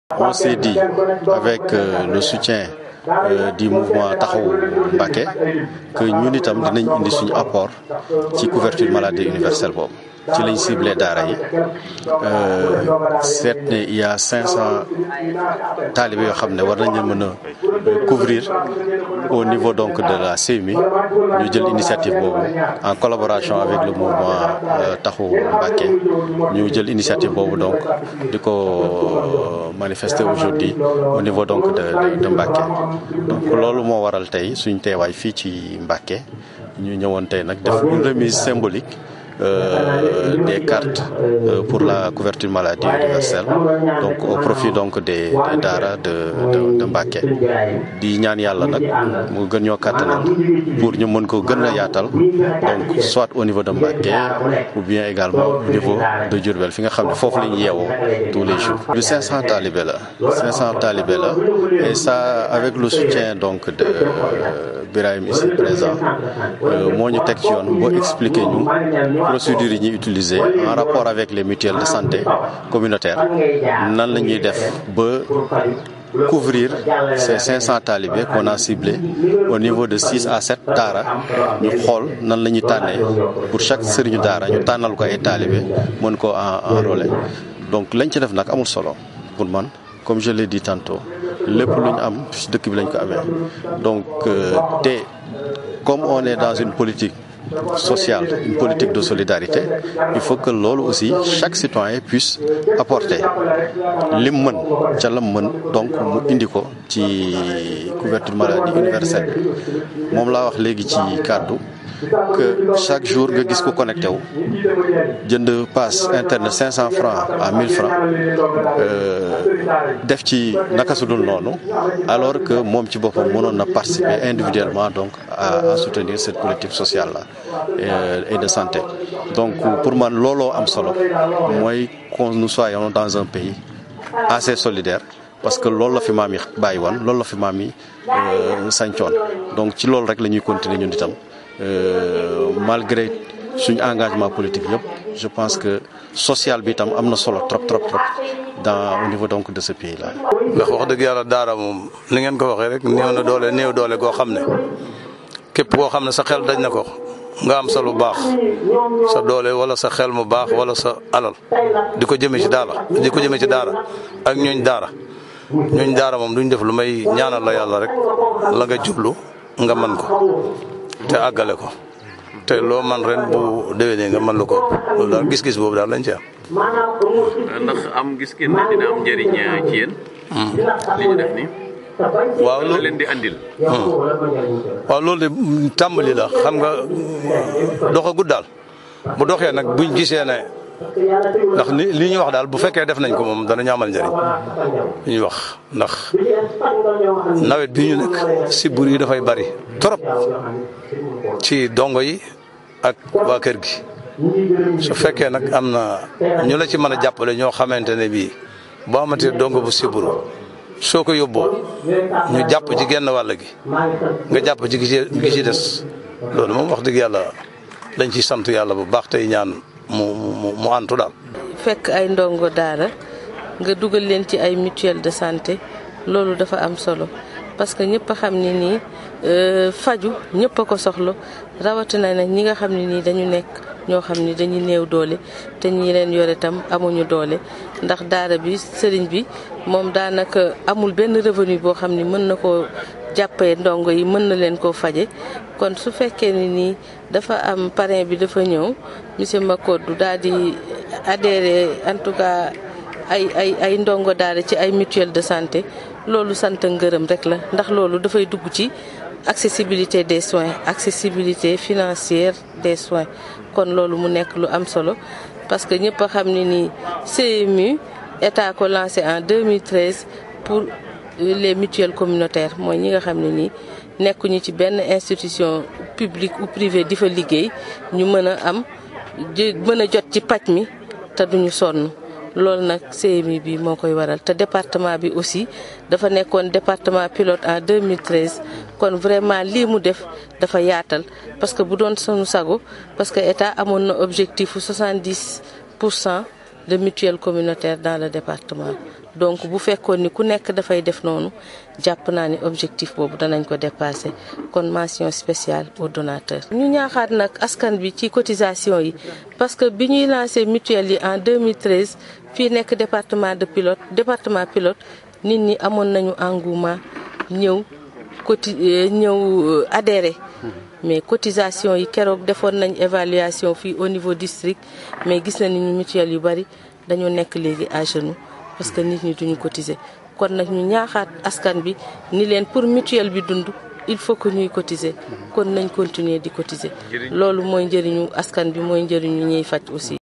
Mbacké: Pape Macodou Diouf inspecteur des impôts et domaines enrôle 500 talibés à la CMU
500 talibés  issus de plusieurs Daraas de Mbacké vont bénéficier de la couverture maladie Universelle. Une prise en charge obtenue grâce à la diligence de M. Pape Macodou Diouf inspecteur des impôts et domaines  chef du bureau régional de Diourbel. Les maitres coraniques saluent le geste qu'ils qualifient de haute portée  sociale. Le médecin chef du district sanitaire de Mbacké qui a part à la remise des kits d'enrôlement à l'école Mbacké Khéwar I s'est de son coté félicité de l'acte qui va permettre à son avis d'aider les maitres coranique qui sont  souvent dépourvue de moyen pour prendre en charge les talibés